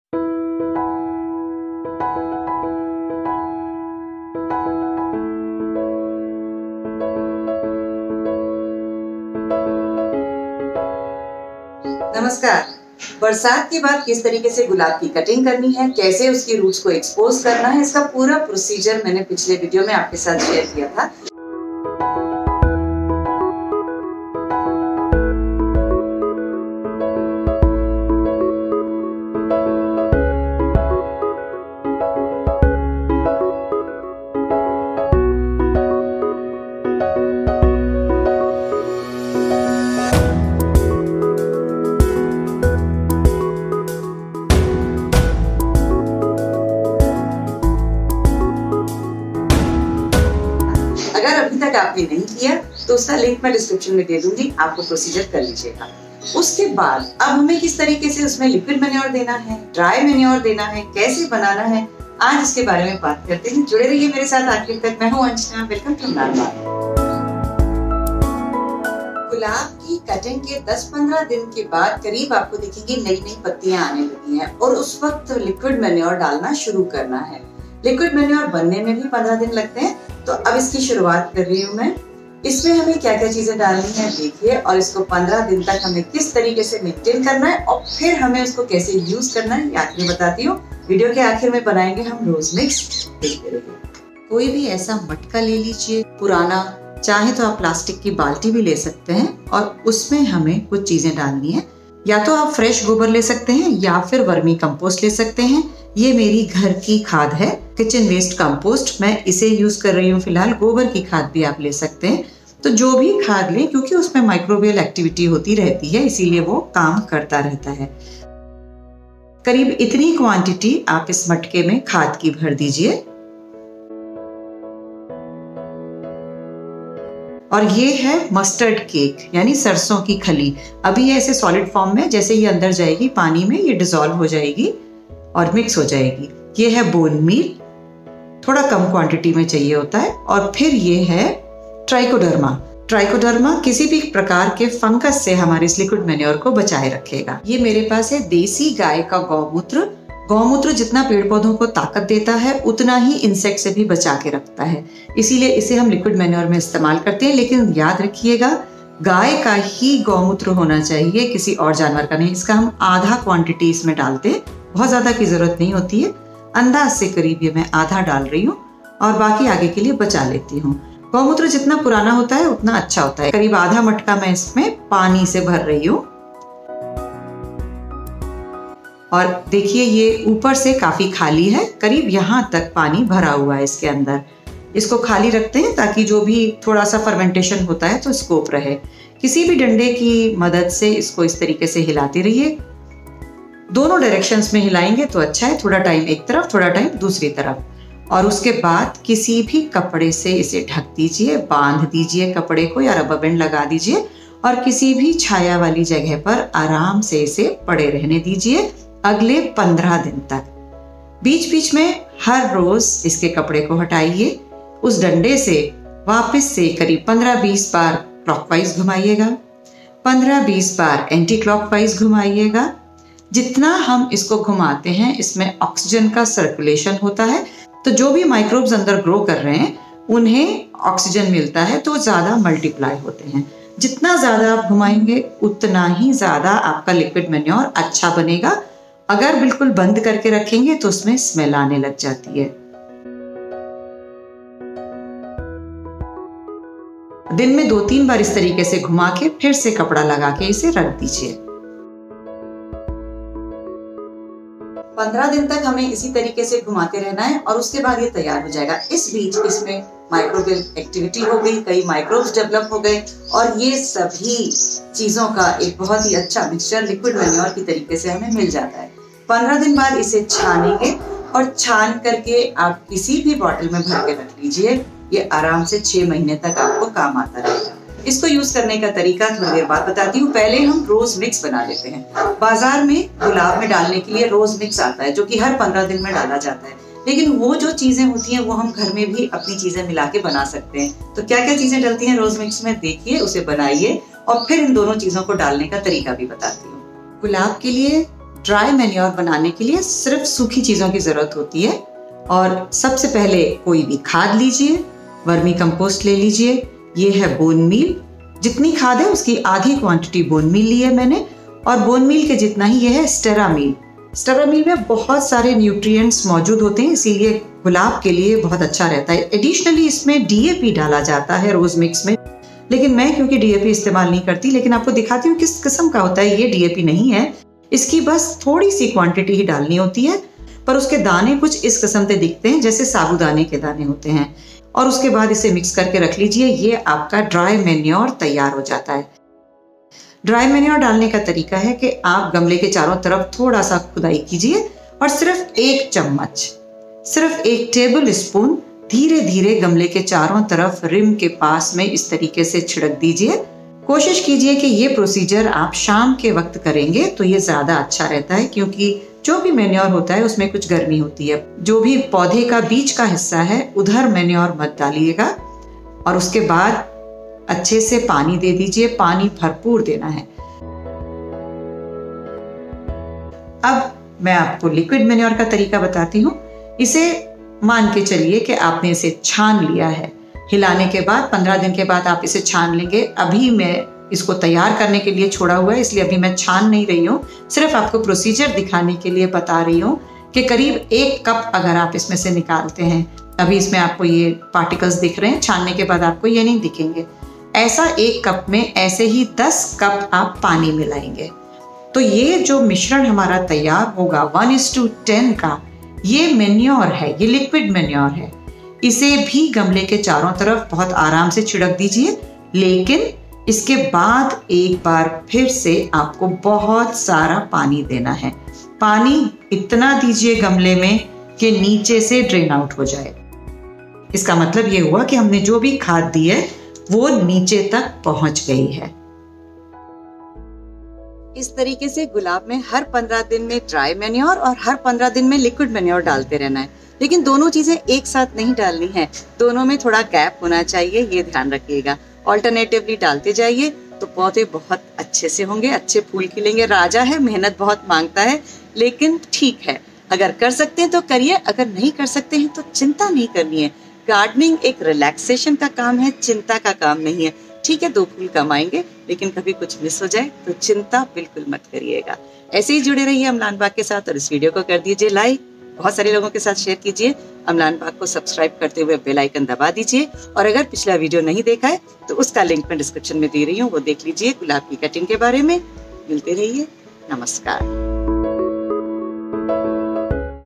Make Your Own Rose Mix & Liquid Manure | गुलाब के लिए ऐसे बनाएं तरल खाद और रोज मिक्स पाउडर
Become a perfect gardener.
Amlaan Baag by Anjana Khilnani